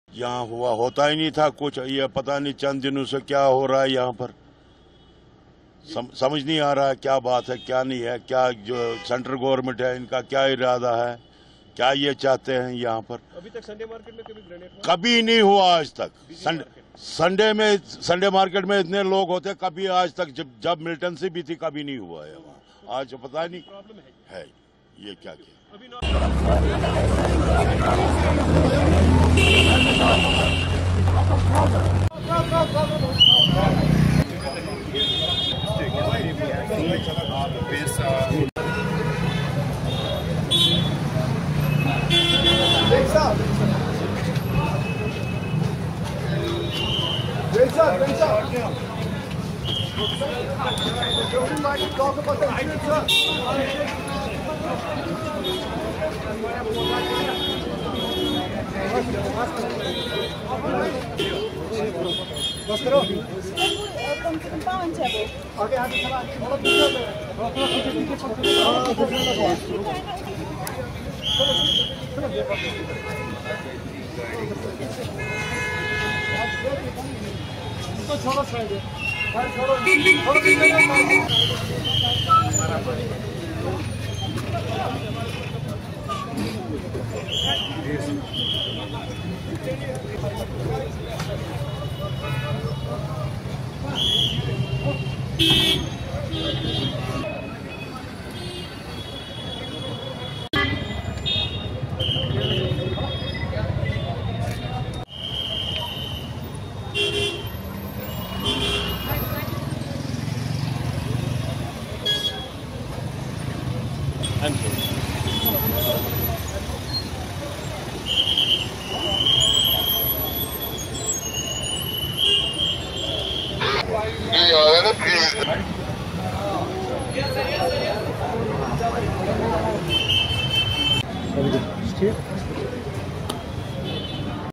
Srinagar Under Attack: "Grenade Attack in Lal Chowk Injures Over 10, Search Operation
A devastating blast has shaken Lal Chowk in Srinagar, leaving more than 10 people injured. The incident has sent shockwaves across the region, and authorities are scrambling to respond.
Eyewitness, "I have never seen a blast like that"